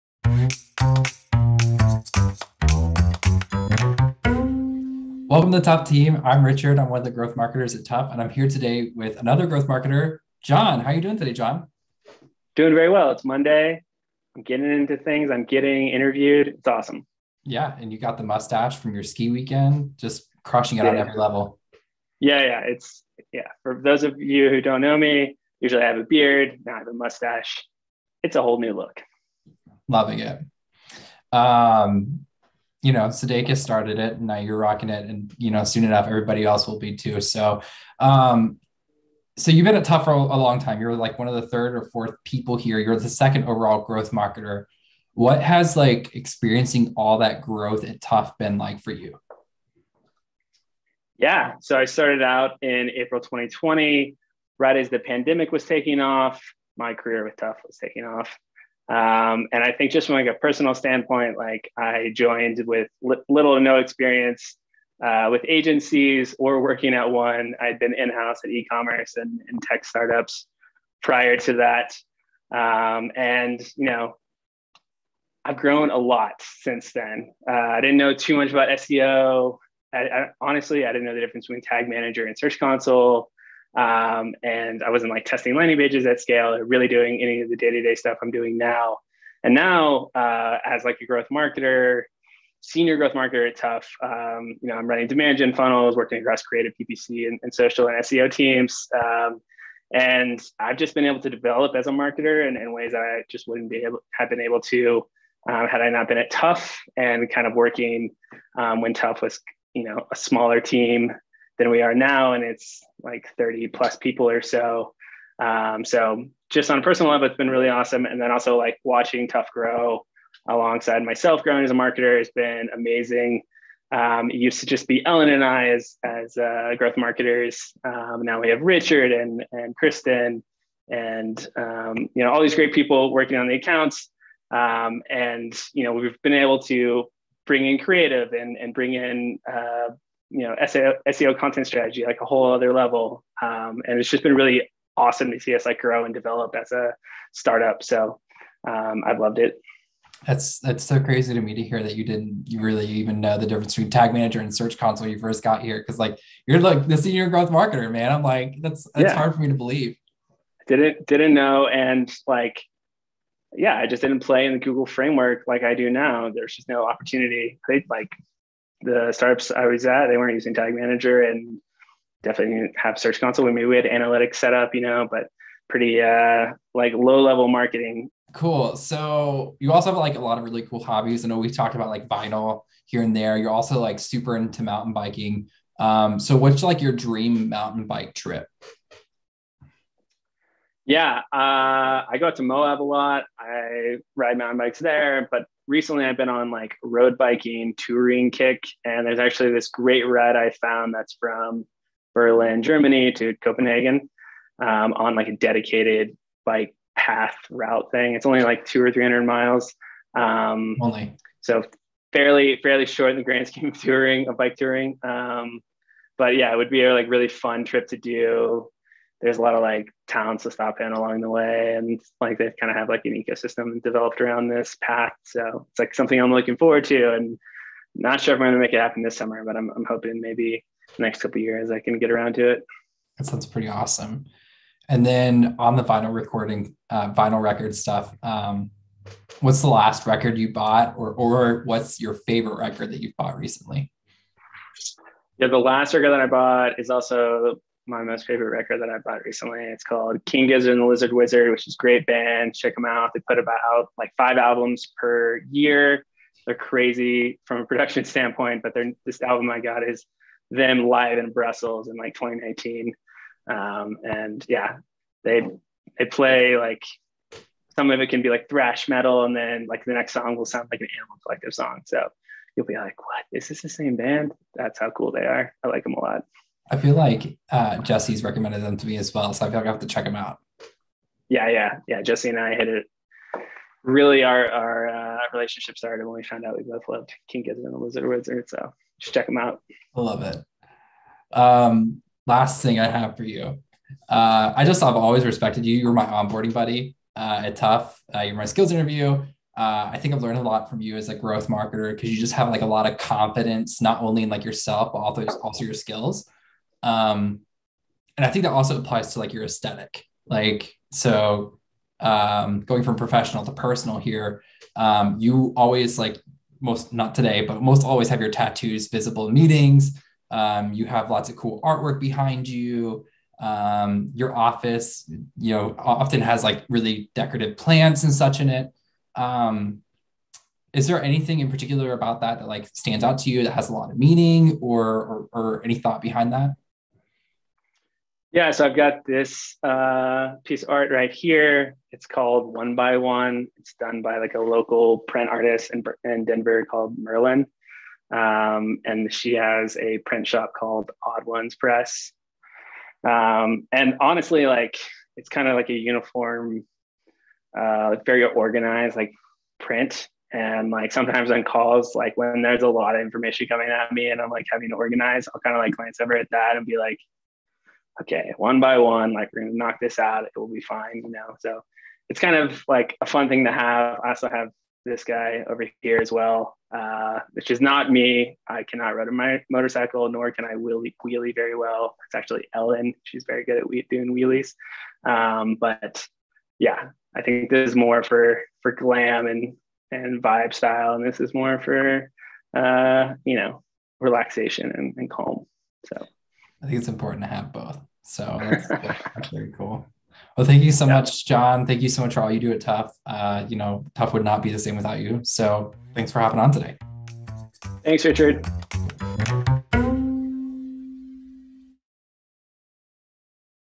Full-funnel growth strategy and great local art with John Atkins, Sr. Growth Marketer
With a fresh new mustache early on a Monday, Richard sits down with John to talk all things Tuff. As the fourth person to join the team, John has been an instrumental part of Tuff’s evolution. Tune in to hear him discuss career growth, explosive learning, and also his ideal bikepacking adventure through Germany and Denmark.